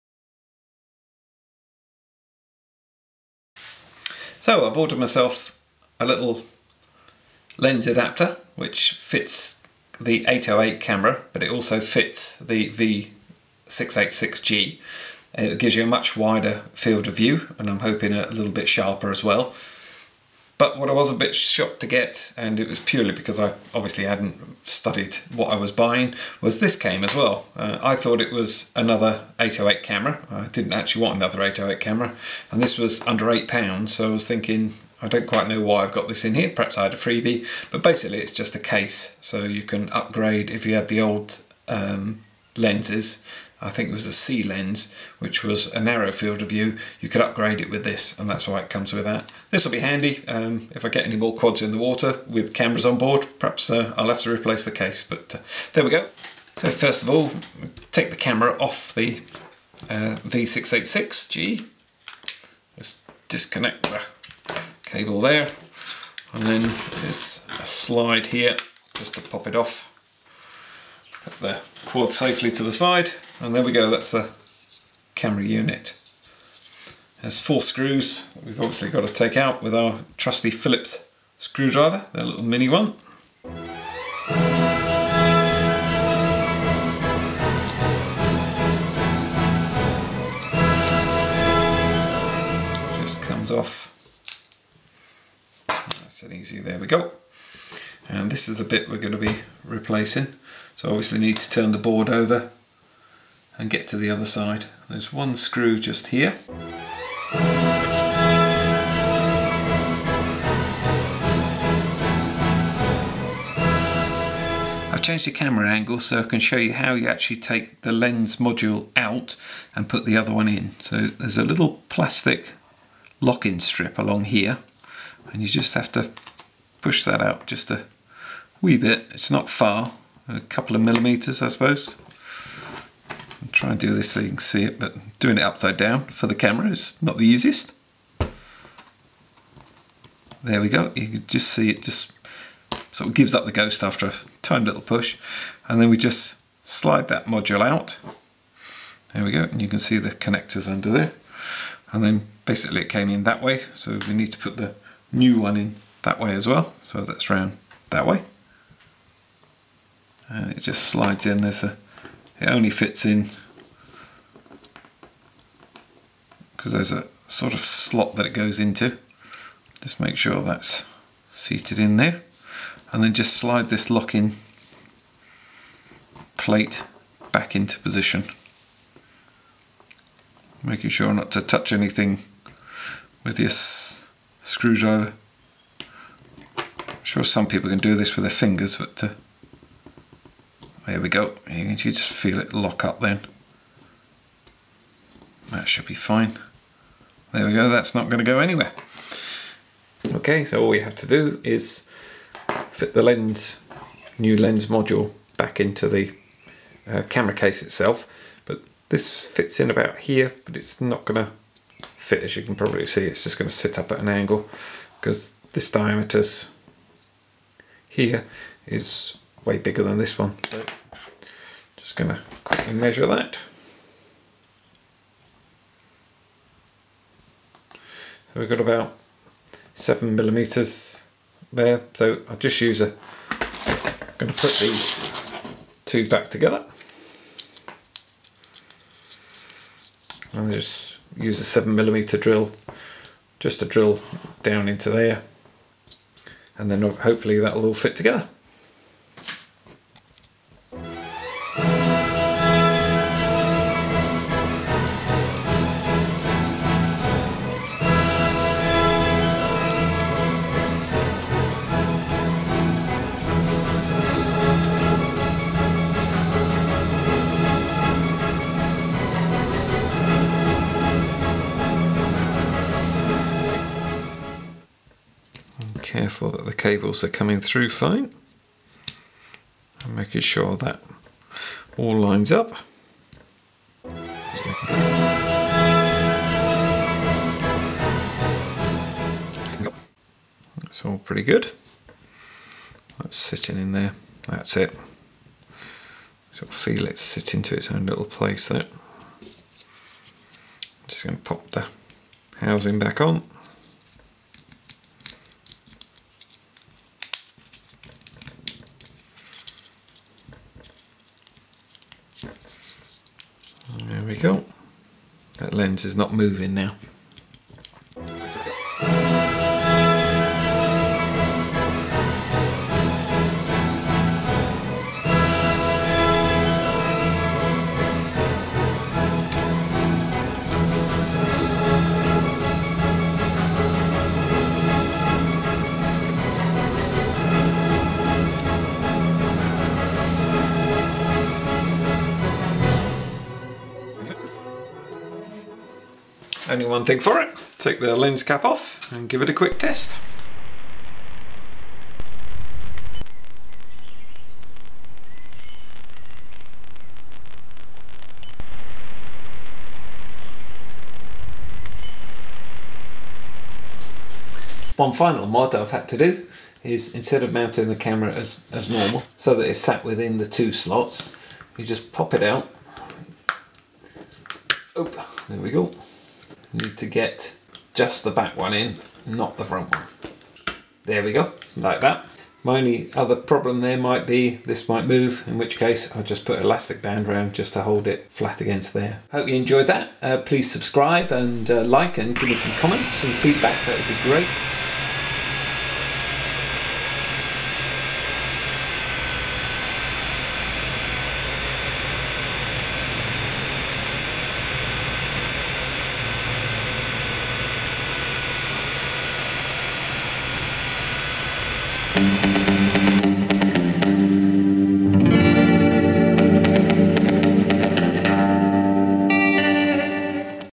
WLtoys V686g replace upgrade wide angle D lens module & test flight
Step by step how to change the original lens module for the 808#16 key-chain camera module giving you a 120 degrees field of view(FOV) with comparison image and flight footage